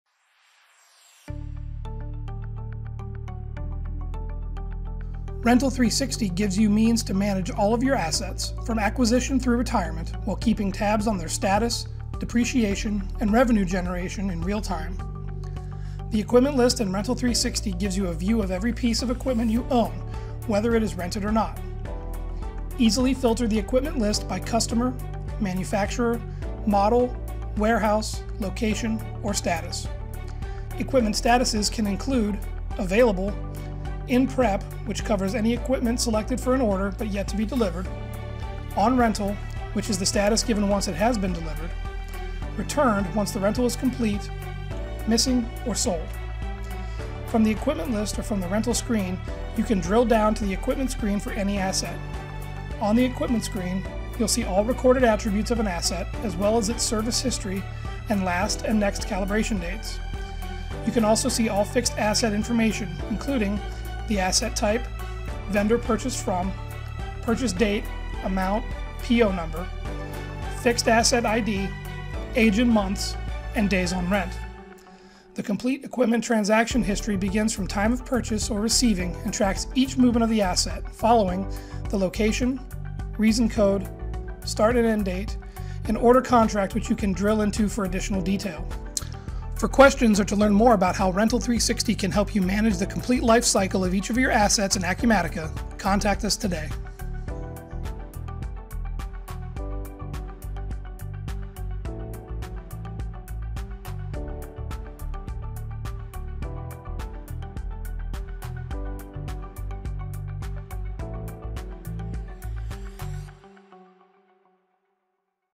Asset Management in Rental360
Discover the Equipment Management/Asset Management capabilities in Rental360 built on Acumatica Cloud ERP software including the Equipment List, Equipment Screen, Fixed Asset Information, and transaction history.